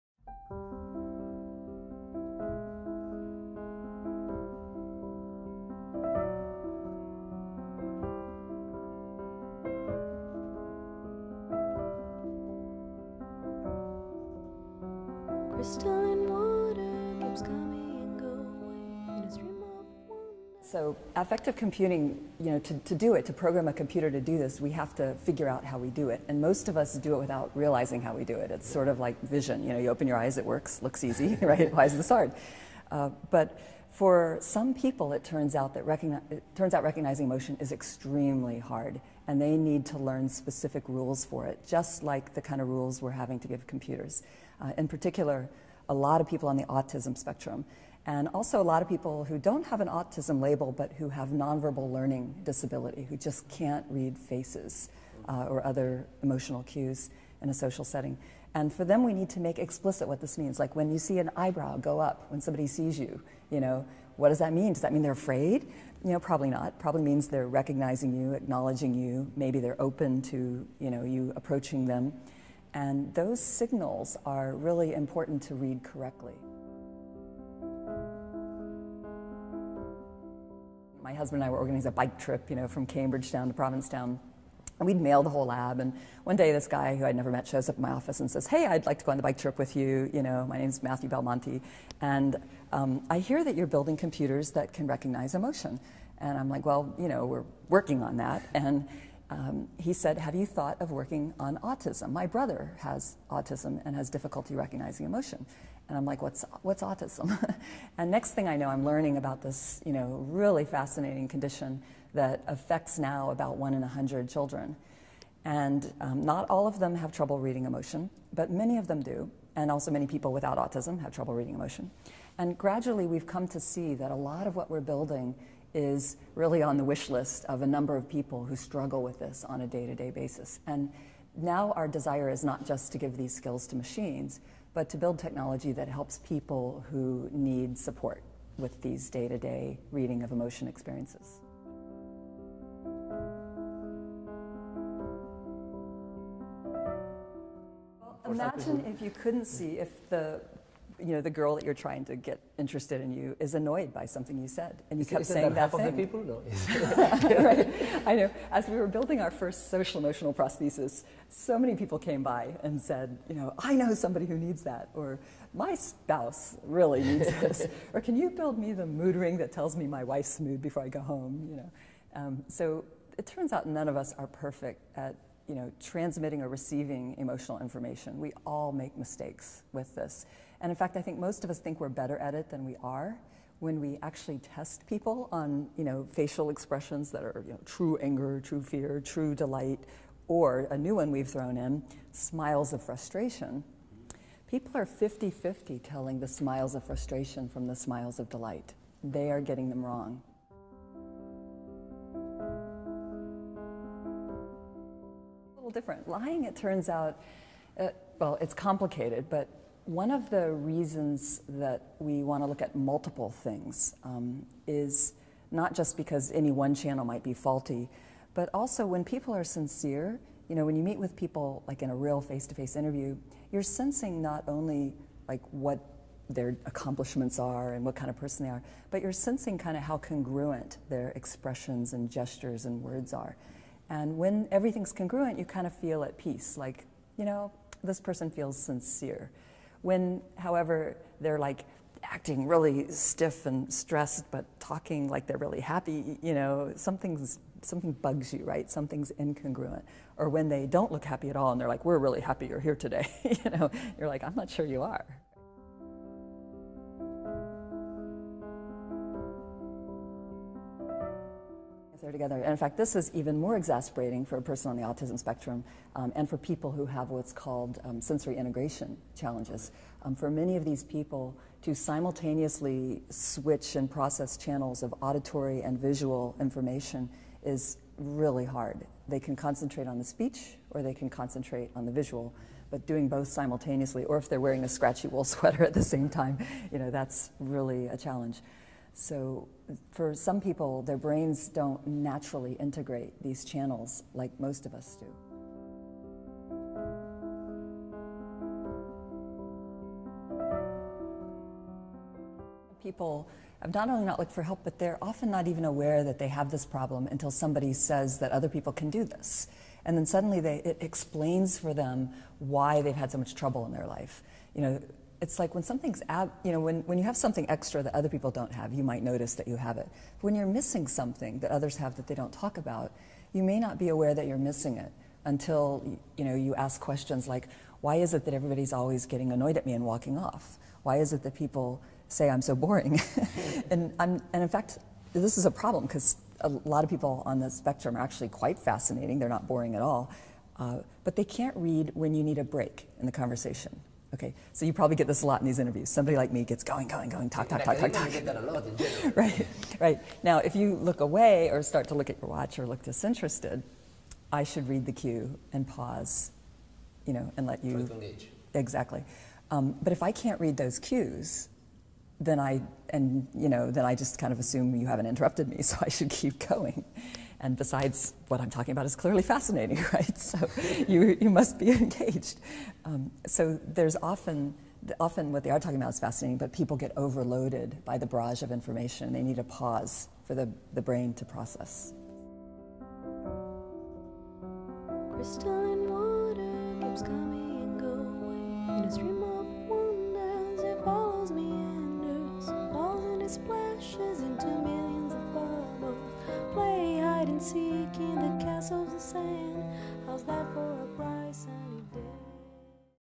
Rosalind Picard - On Emotions and Autism
Rosalind Picard talks to us about emotions and autism. Roz is a Professor of Media Arts and Sciences at MIT, director and also the founder of the Affective Computing Research Group at the MIT Media Lab, and co-director of the Things That Think Consortium.
1. How can affective computing improve people's ability to understand emotions?
2. How has this research been used in autism?
3. How is life difficult for people that have trouble reading emotions?
4. Then, is it easy to lie?
5. How do these multiple channels affect autistic people's ability to understand emotion?
6. Are people aware of their ability or inability to read emotions?